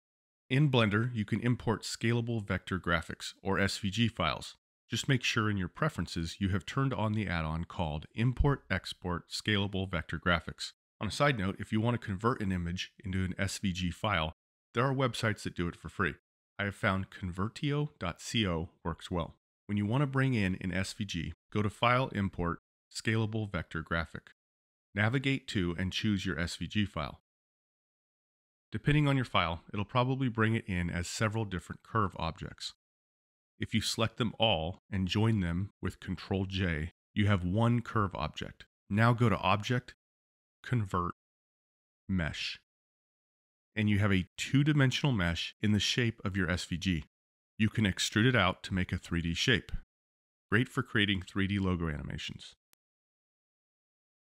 Turn SVG image files into 3D meshes in Blender!
Blender allows you to import SVG files. Standard Vector Graphic (SVG) files import into Blender as curves and can be incredibly useful in creating animated 3D logos. This quick Blender tip will show you how to import an SVG file into Blender and convert it into a mesh object. I'll also show you where you can convert images to SVG files using Convertio.co.